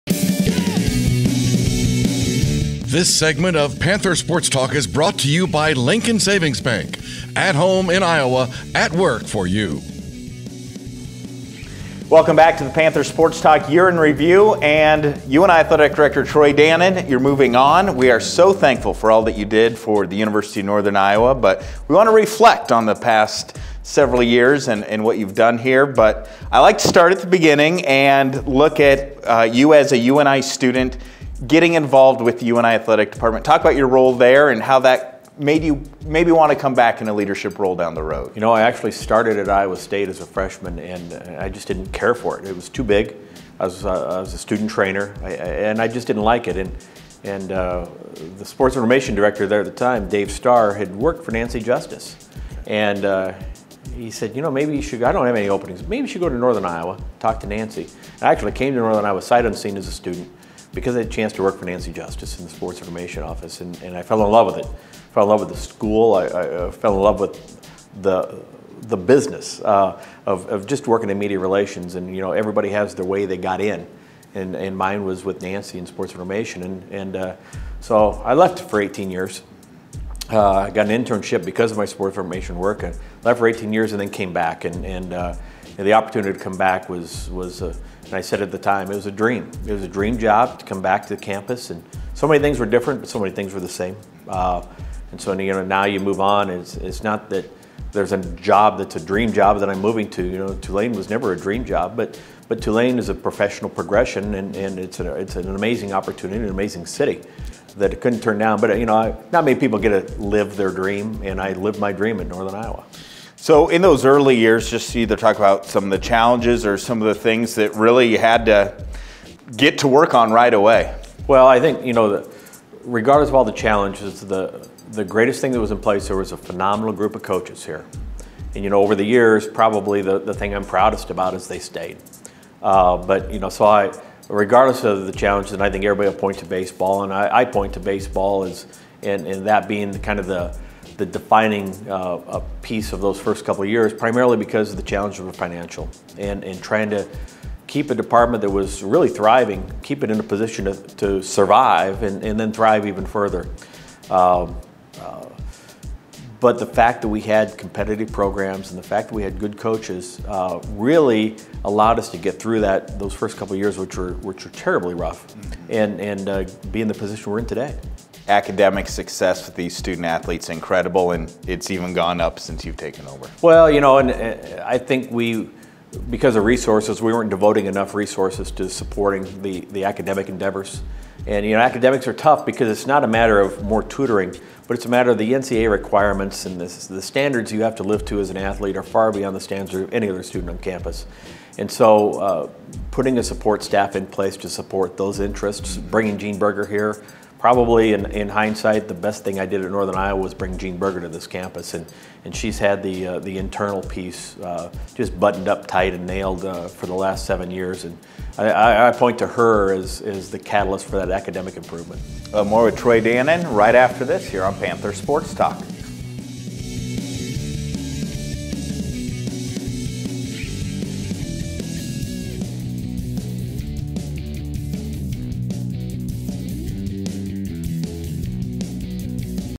Troy Dannen reflects on his 8 years at UNI
Troy Dannen reflects on his 8 years at UNI as Director of Athletics.